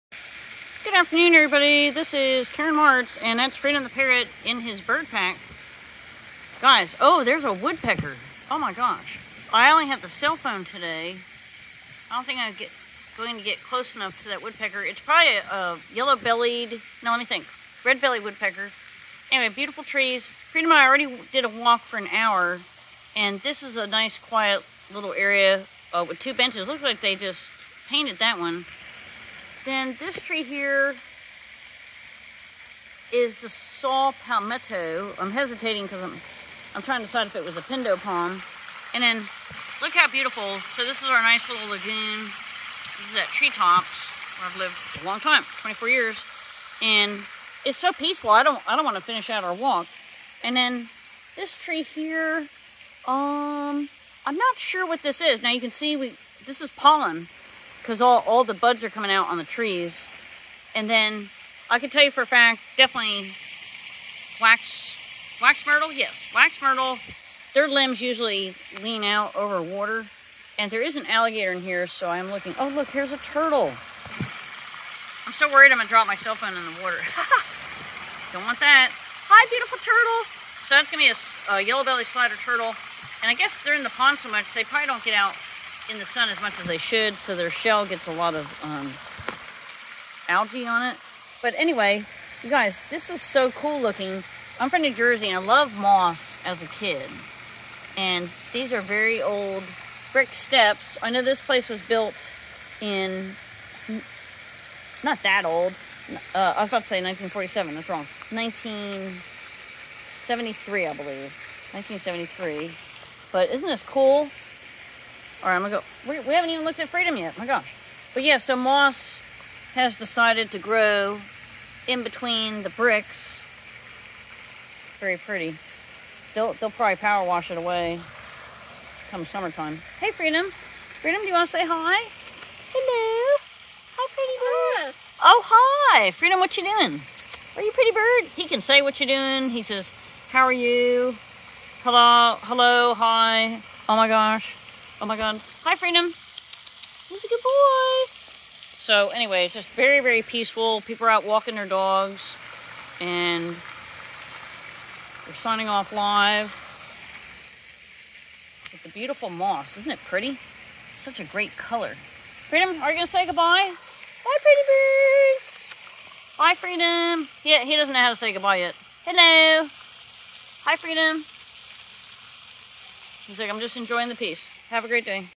Freedom the Parrot & Yellow Bellied Slider Turtle by Moss Lined Brick Deck at Treetops! Hilton Head
Freedom the Parrot & Yellow Bellied Slider Turtle by Moss Lined Brick Deck at Treetops! Hilton Head Island, SC